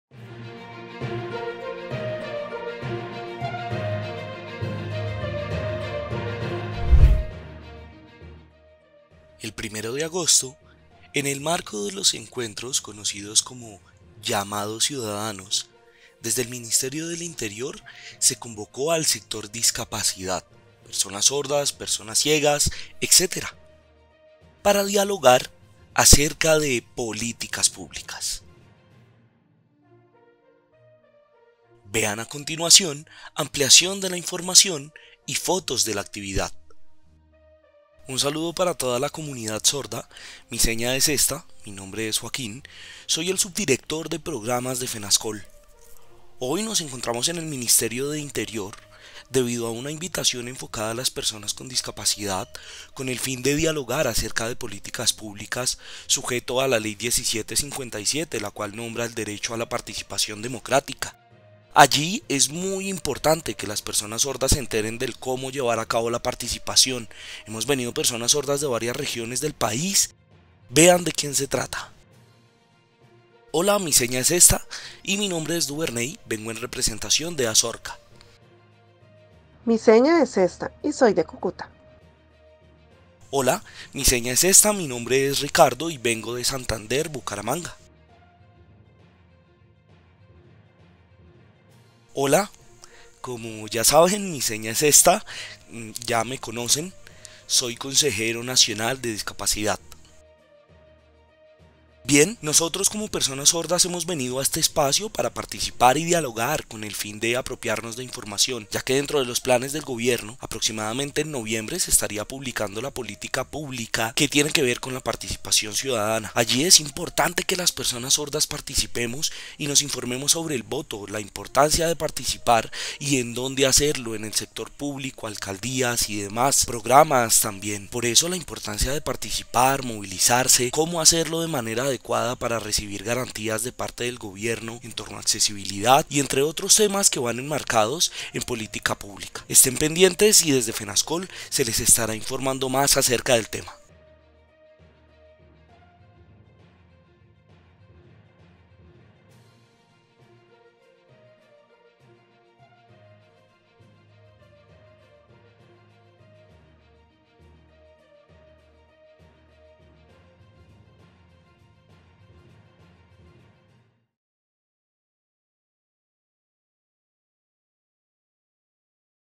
NotiSeñales Ministerio del Interior en el marco de los encuentros conocidos como Llamados ciudadanos
El 1 de agosto, el Ministerio del Interior en el marco de los encuentros conocidos como "Llamados ciudadanos - Política Pública de Participación Ciudadana y Electoral" invitó a las personas Sordas a la actividad que te compartimos en este video.